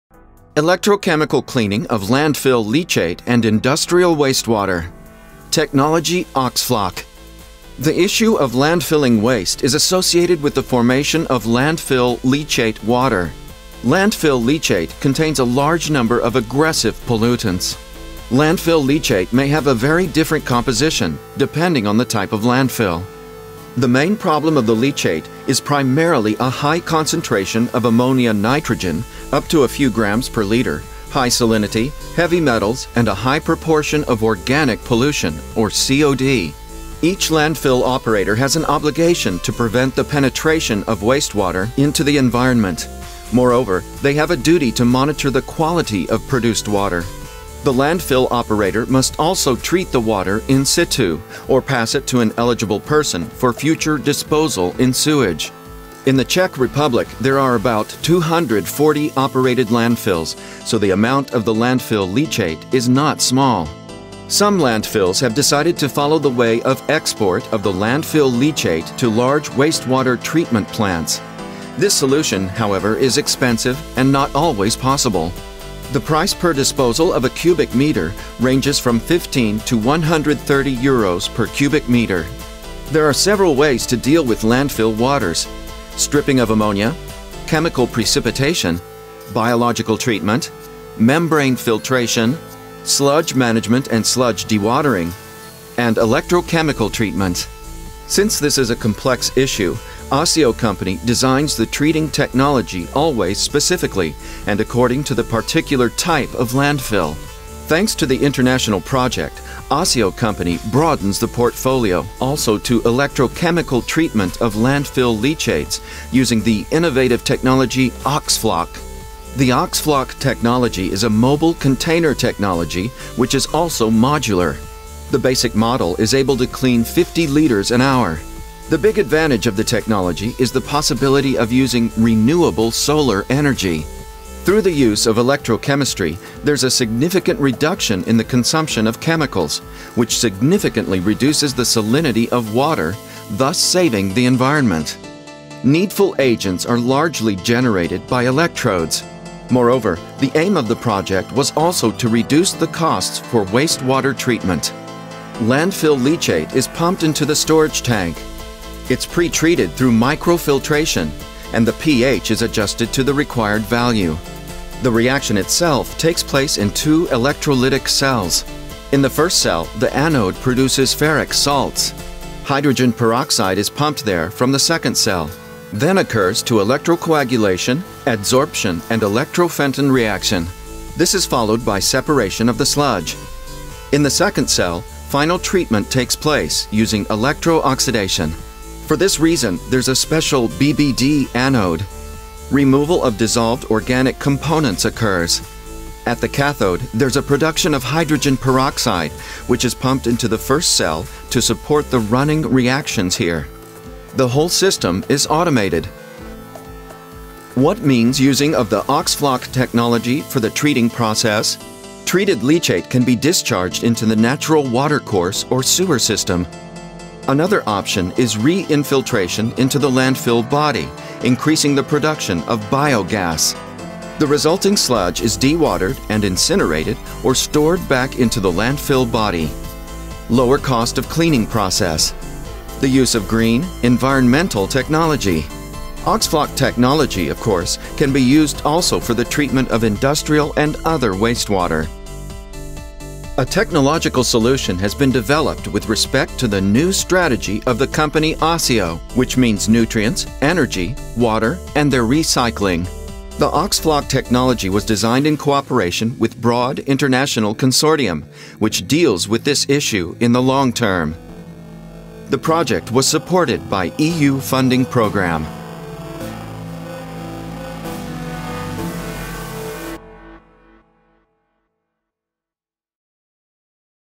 OxFloc _ Reducing of the cost of wastewater treatment from industry
The OxFloc project aim´s to reduce the cost of wastewater treatment from industry at least 30%, while offering modular installation and application of engineering technology at a lower price than comparable existing processes. OxFloc systems will have a clearly defined environmental benefits, smaller size, lower operating costs and allows 100% recycling of water. Furthermore, the system does not involve chemicals and salts.